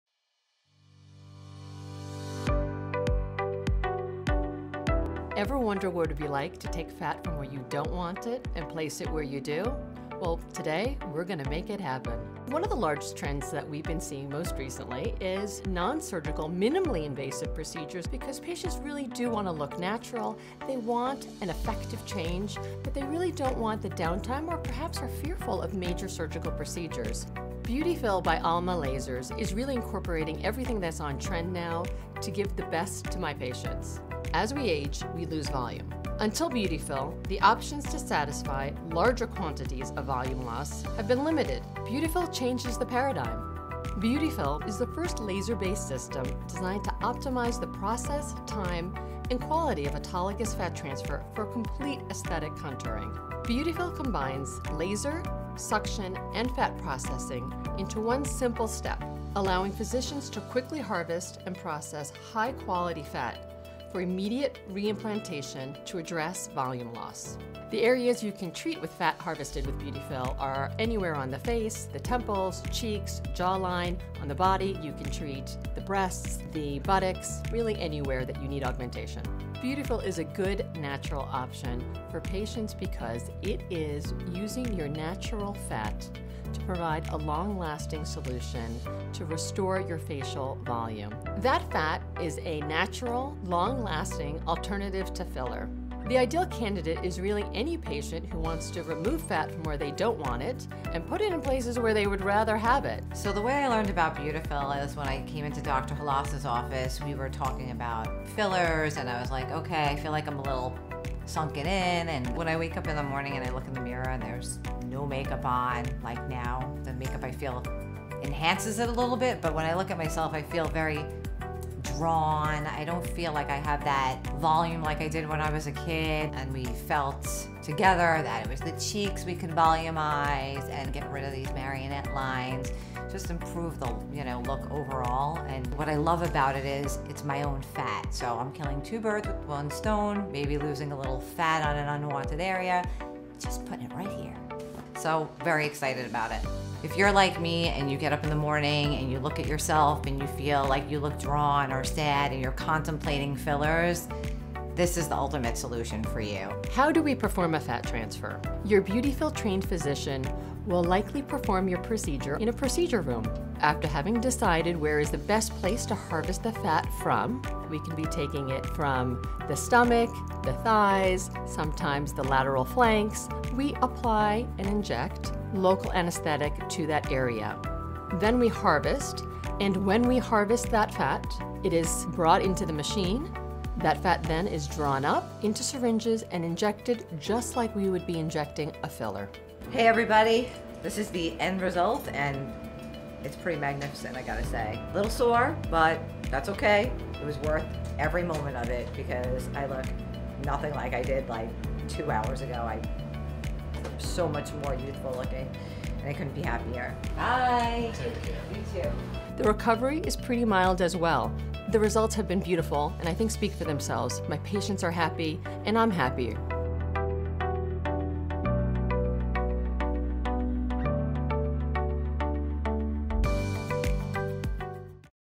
BeautiFill Patient Testimonial Video
It happens to all of us - we lose volume as we age. Wouldn't it be great if we could take fat from where we don't want it and use to restore the vibrant look of our youth? BeautiFill makes it possible and Irina, a BeautiFill patient, is happy to share her story.